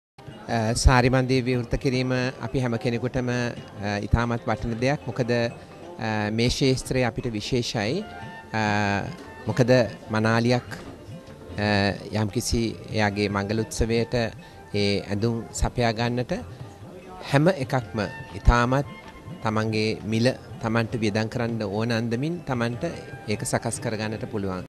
Harris, Fashion Designer
Comments from Mr. Harris Wijesinghe, Fashion Designer
Saree Mandir, Sri Lanka - Opening Ceremony